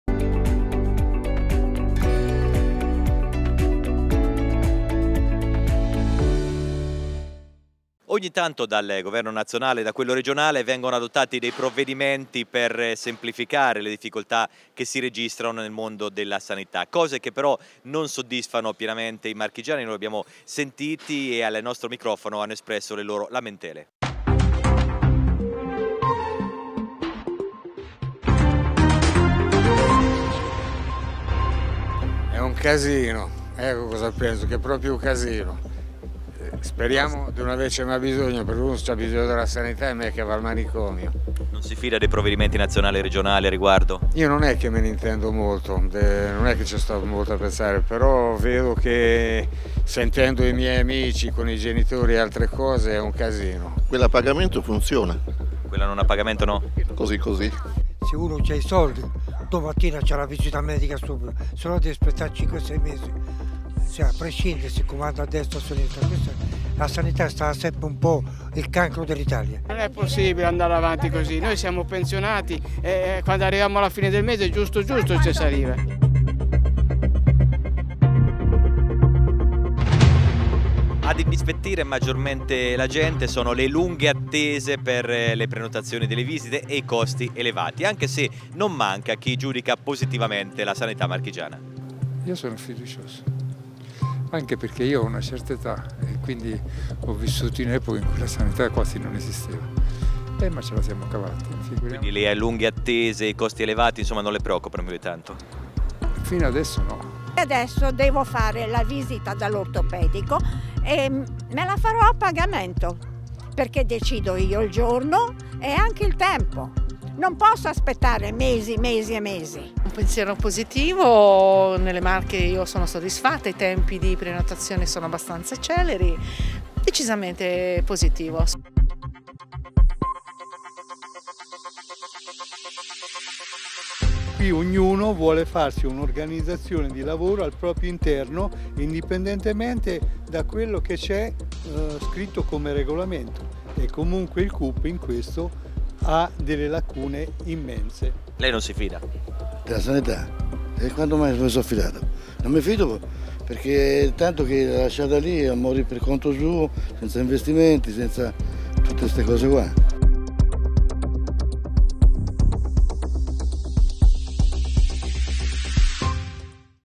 Ancona - Sanità, marchigiani insoddisfatti: "Attese e spese esagerate"
Nonostante i provvedimenti del governo nazionale e regionale per rendere meno complicata la gestione della sanità, la gente è insoddisfatta